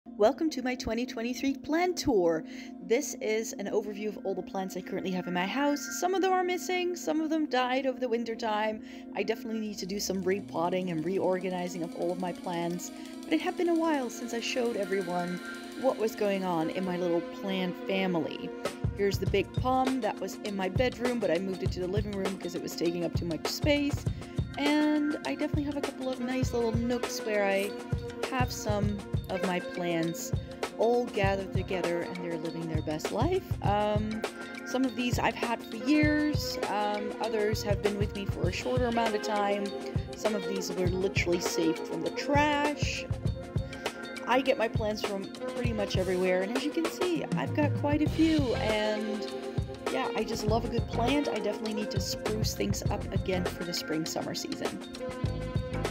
PLANT TOUR 2023
Plant Tour 2023!!! It's been a while since I showed you my plant family. They are in desperate need of some sprucing up in some cases, but I am looking forward to spring and rehabilitating my plants after their winter slumber.

New reviews go live Monday – Wednesday – Saturday